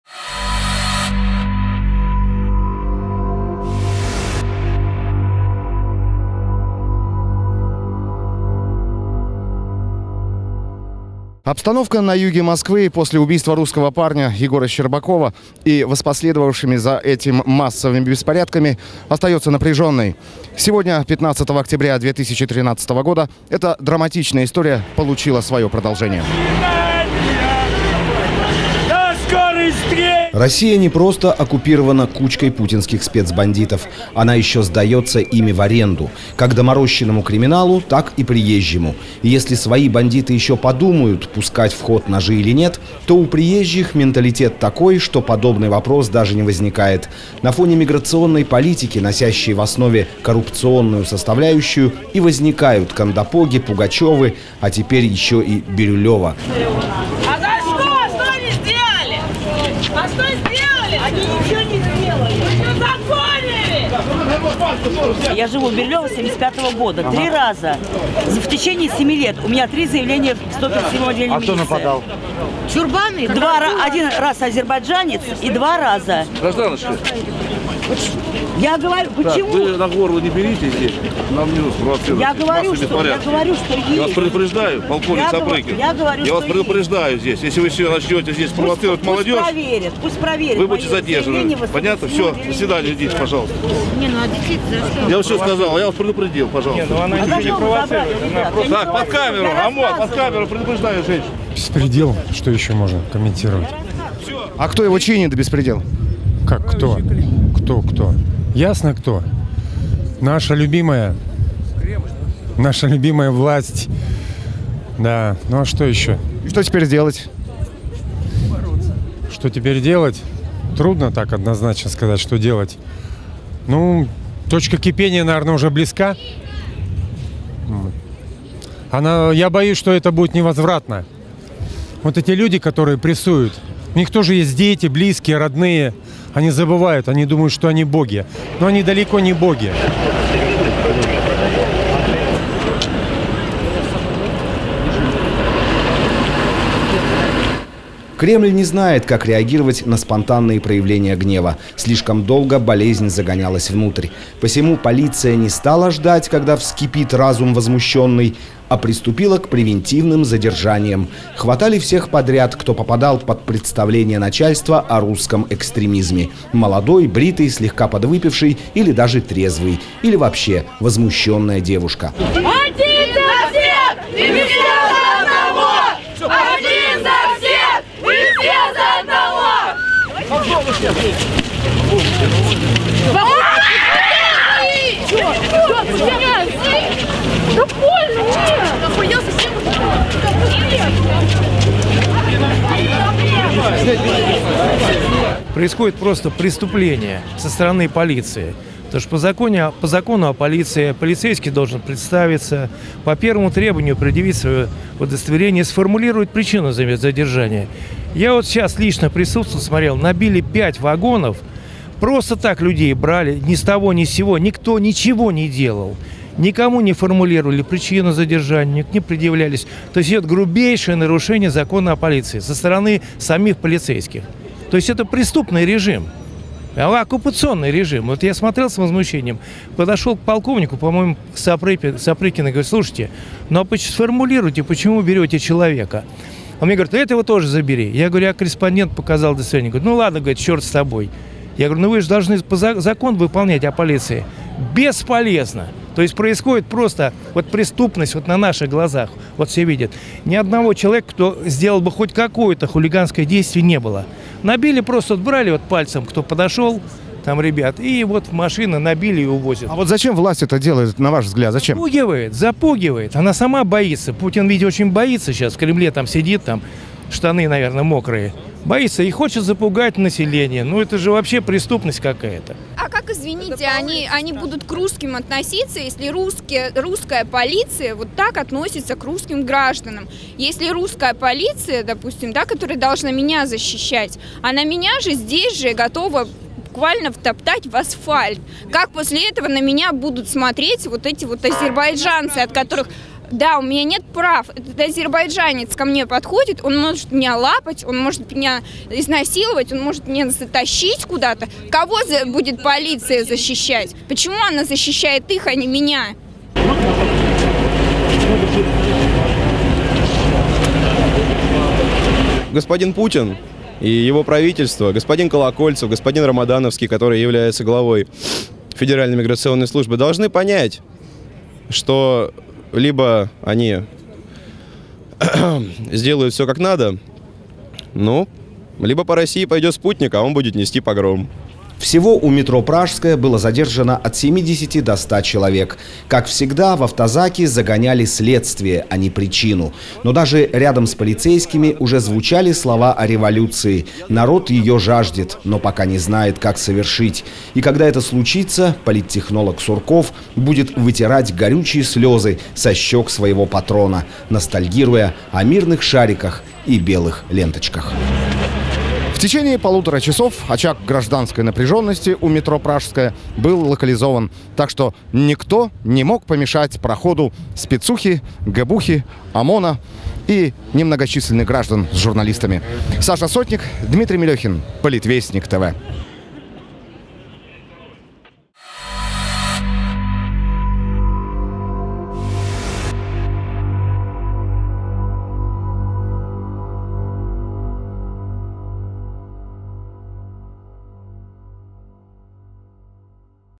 БИРЮЛЕВСКИЙ КАПКАН. ЗАЧИСТКА У МЕТРО ПРАЖСКАЯ
15 октября 2013 г. Бирюлевская история получила свое продолжение, невзирая на объявленную поимку убийцы Егора Щербакова. У станции метро Пражская собрались граждане, недовольные миграционной политикой Кремля. Полиция произвела массовые задержания. Смотрите репортаж Politvestnik.tv